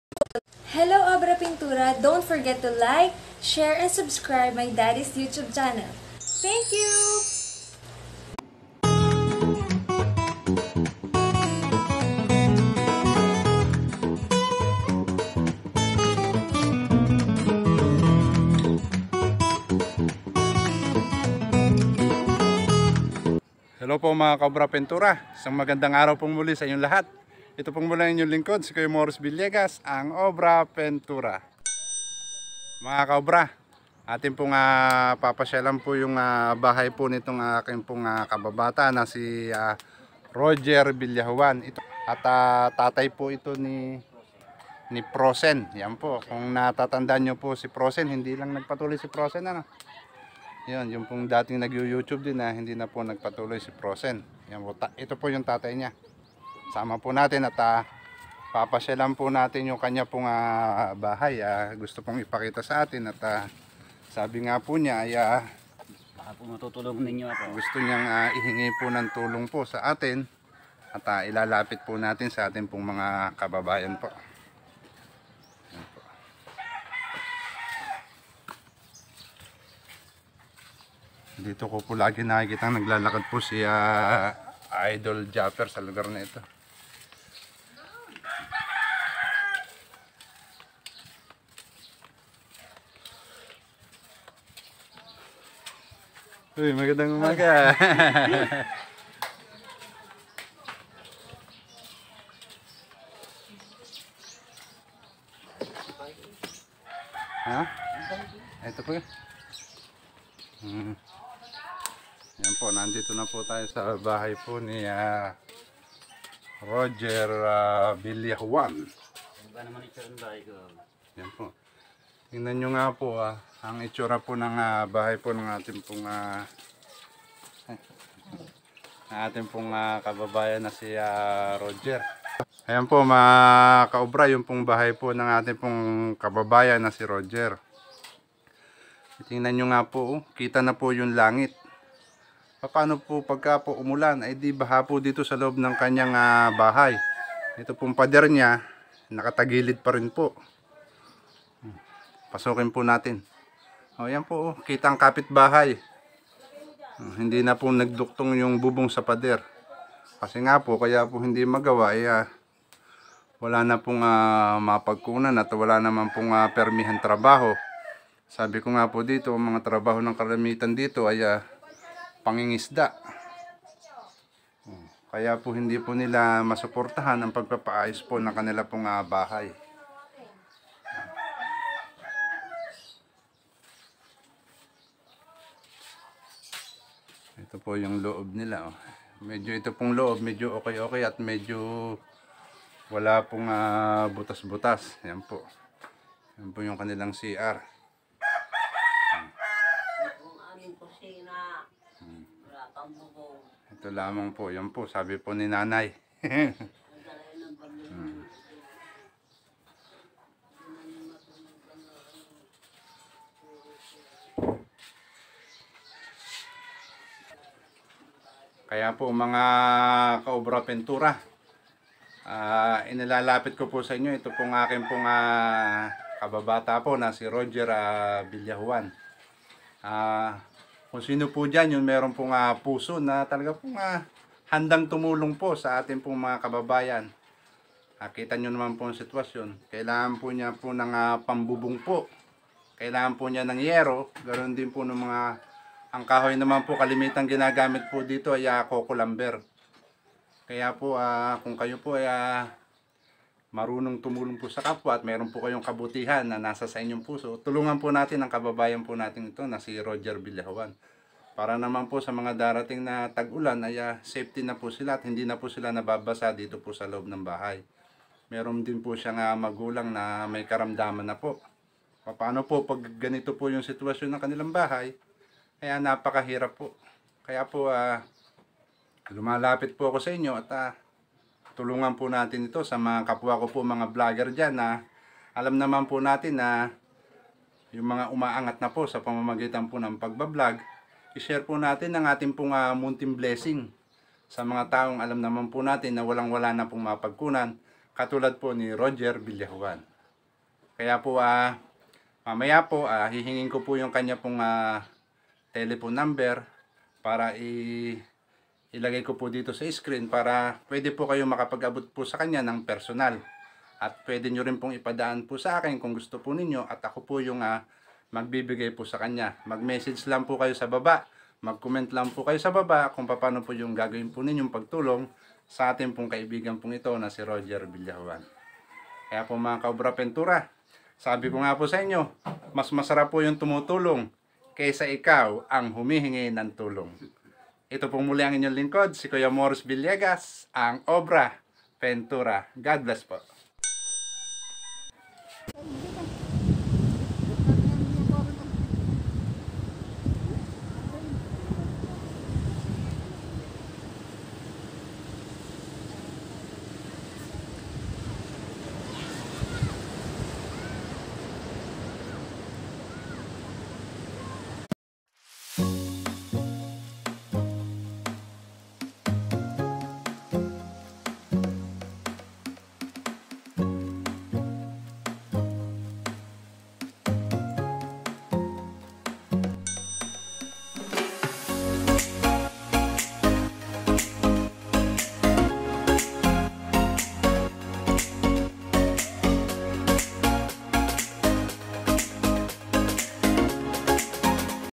FROZEN OFFICIAL |BAHAY NI ROGER VILLAJUAN || By: Morris Villegas
Si Roger Villajuan ay isa sa aking matalik na kaibigan nuong kami'y nanirahan sa Brgy. Bucana, Nasugbu Batangas.

how to start an organization to help the poor
what is poor organization